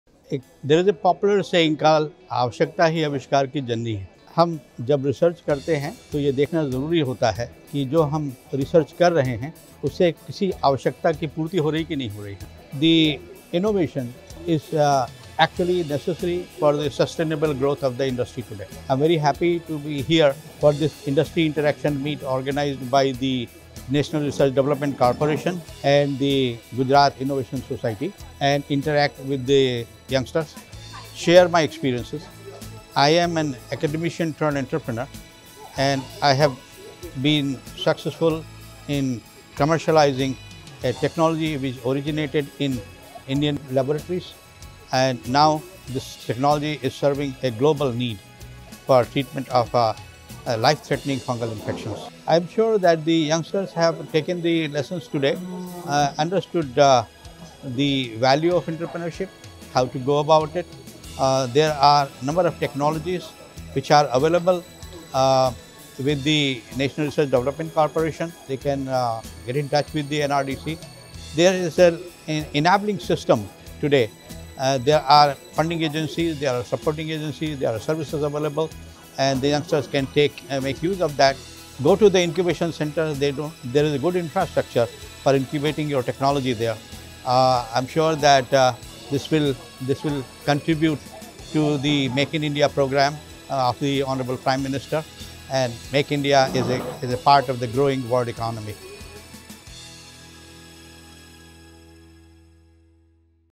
Innovation is necessary for susceptible growth - Mr. Jitendra Verma
Innovation is necessary for susceptible growth.

Mr. Jitendra Verma
Founder & Director, Fine Care Pharma